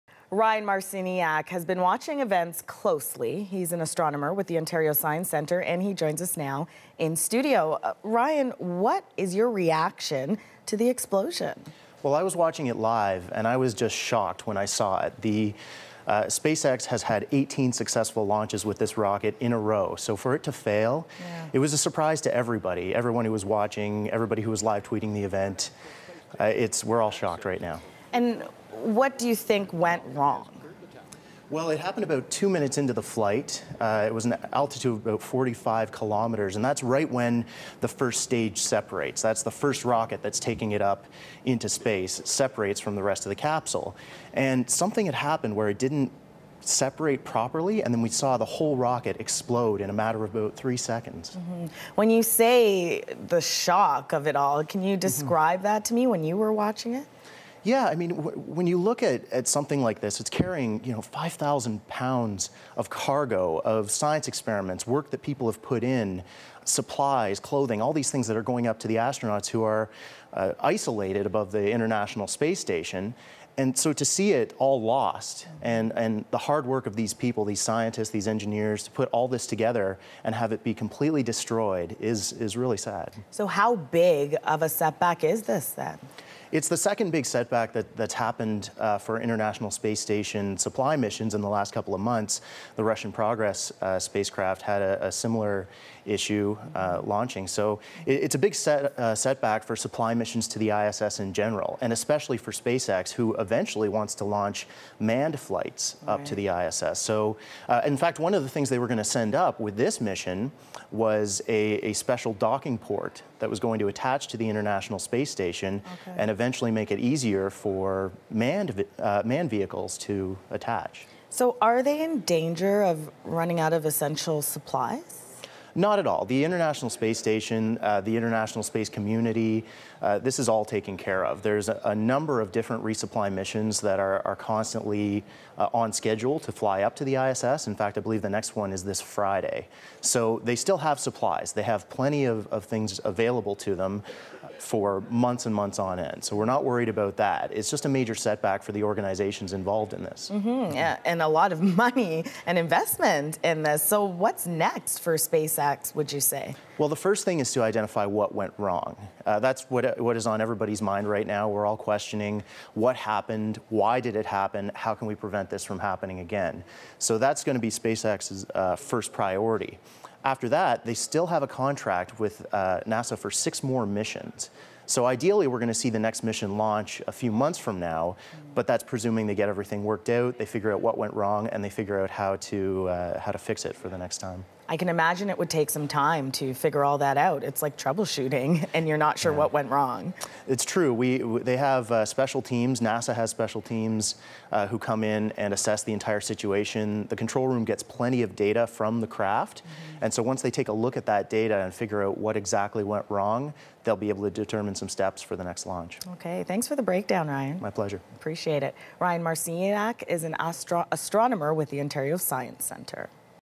Astronomer on SpaceX rocket failure
'SpaceX has had 18 successful launches with this rocket in a row. So for it to fail, it was a surprise to everybody,' says Ontario Science Centre's Ryan Marciniak